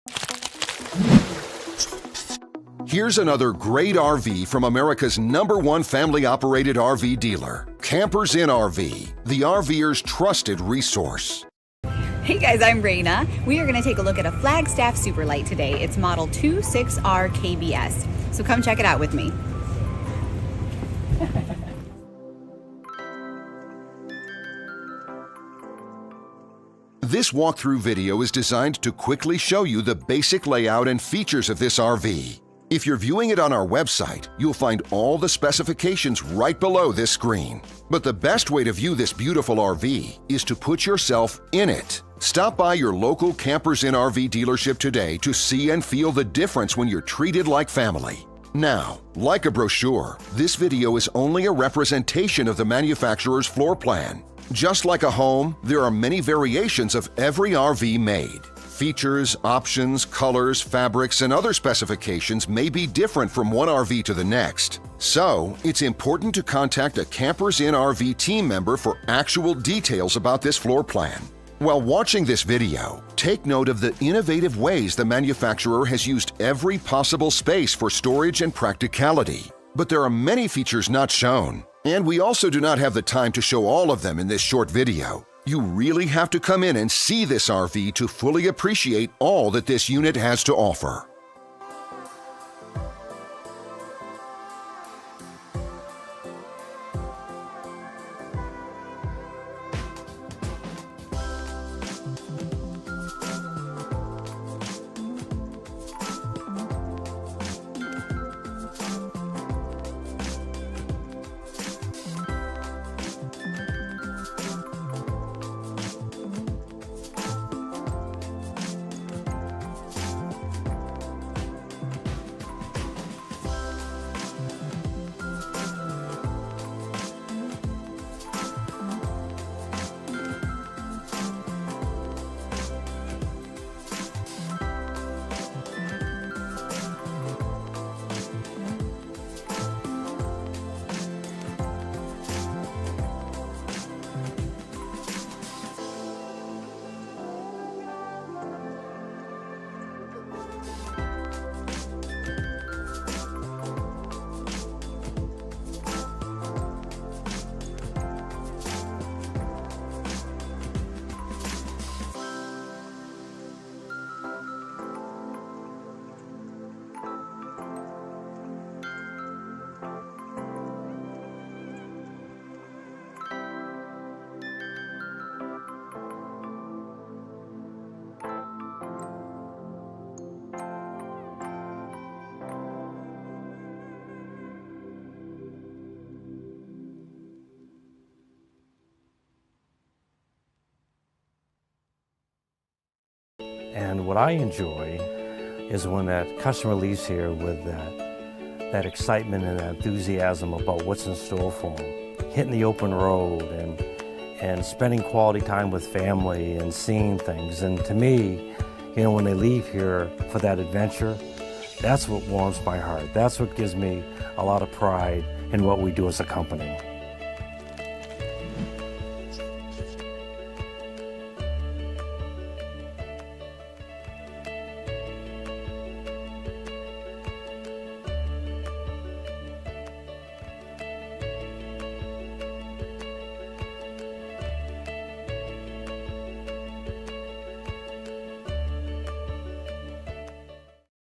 Forest River-Flagstaff Super Lite-26RKBS - by Campers Inn RV – The RVer’s Trusted Resource
Forest River Flagstaff Super Lite travel trailer 26RKBS highlights:
Dual Entry
Rear Kitchen
Hide-A-Bed Sofa
21' Power Awning
Outside Kitchen
Whether you camp at state parks, National forests or on your friend's land, this trailer offers you plenty of floor space thanks to the two slide outs!  As you enter the main entrance near the rear kitchen you can grab a drink from the refrigerator or a snack from the pantry and relax on the hide-a-bed sofa or sit at the booth dinette. Both of the furnishings are within the slide out and provide sleeping space for family and friends.  The LED TV offers entertainment while the fireplace will keep you cozy during the cooler months. Everyone will appreciate having a full bathroom to get cleaned up in, and the front bedroom gives you privacy and a queen bed, plus storage and the second exterior entry.
With any Flagstaff Super Lite travel trailer by Forest River the towing features include the TST tire pressure monitor system with a monitor providing safety on the road, Easy Lube axles and Nev-R adjust brakes, a power tongue jack for easy setup, and rear observation camera prep. Each is built with 6 sided aluminum framed side walls, end wall, front, floor and a roof, a plywood tongue and groove subfloor, electronically controlled heated holding tanks and an enclosed underbelly allowing you to travel during any season. The interior offers a Water-Pur filtration system, a multi-zone stereo with DVD, CD, AM/FM and Bluetooth control, a Showermiser water saving system, designer bedding and so much more! Choose a lightweight, towable by half-ton pickup truck Flagstaff Super Lite today!

38 Locations and Growing!

NEW JERSEY
Dyl